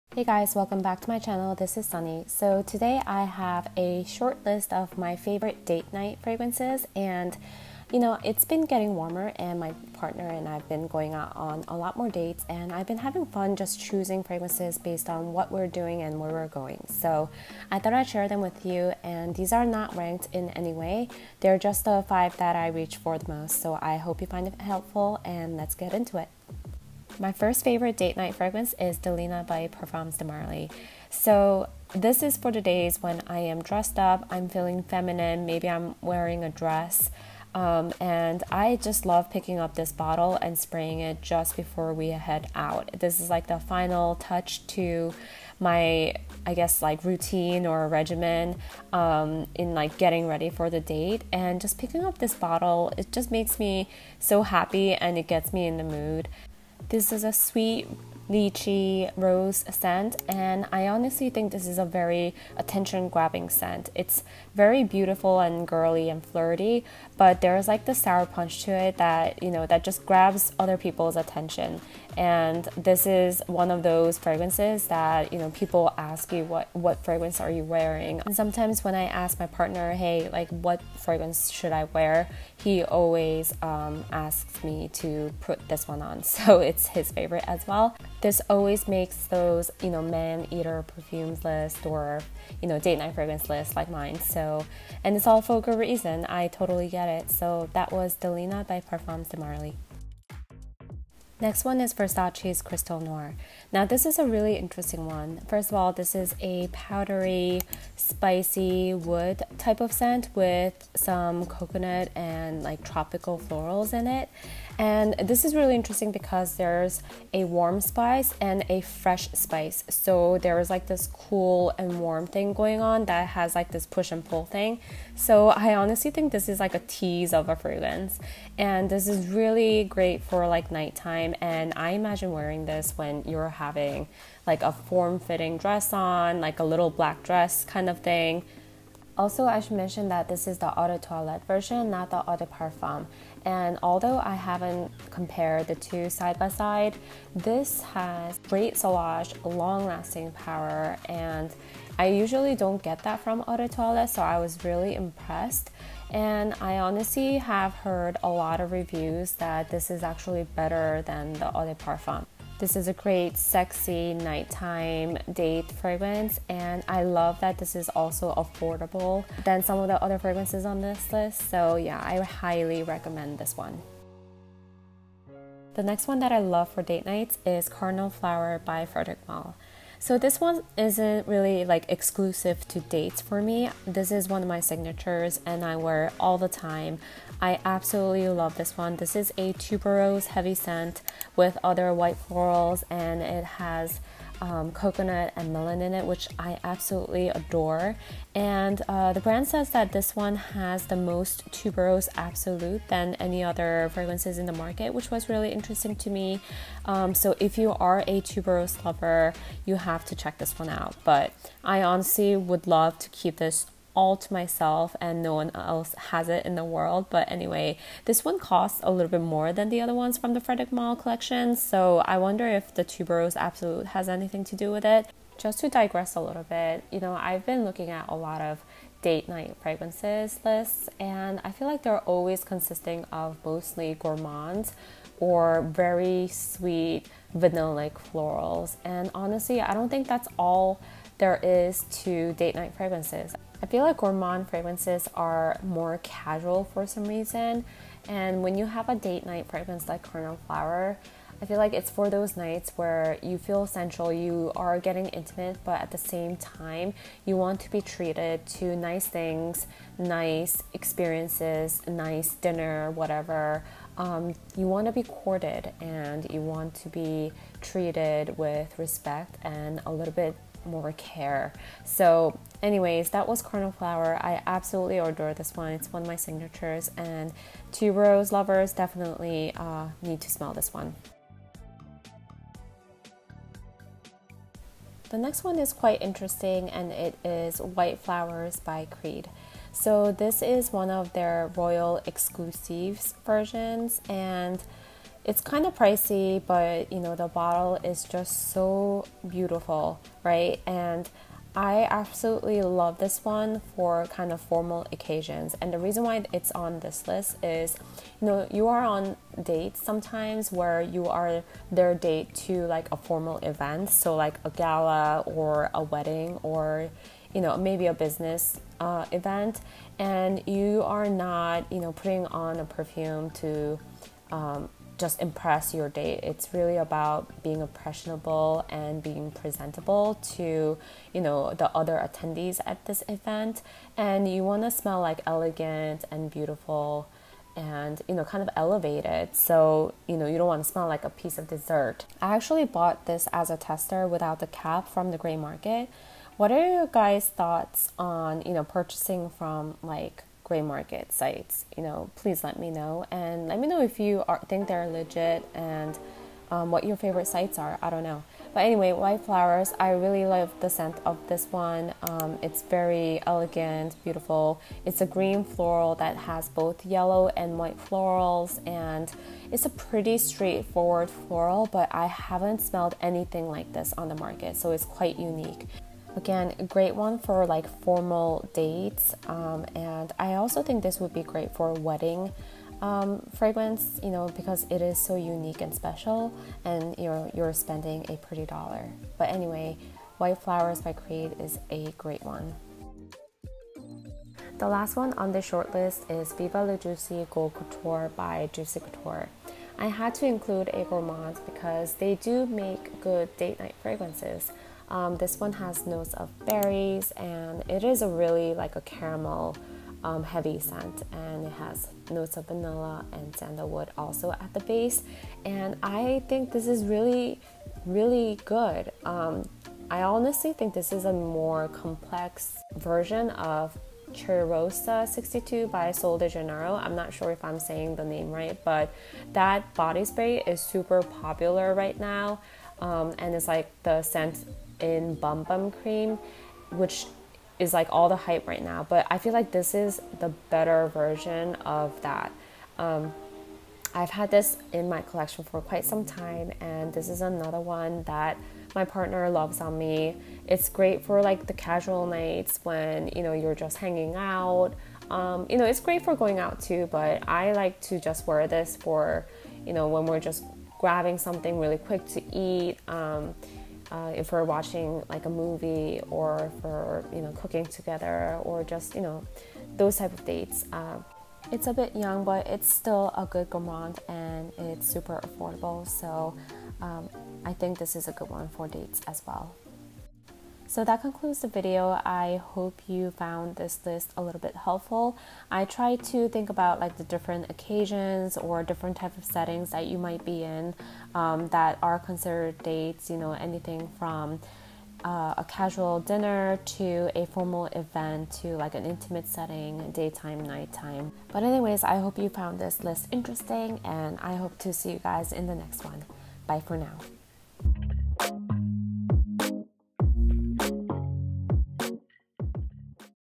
Top 5 Women's Fragrances for DATES
These are the five I reach for the most when getting ready to go on dates.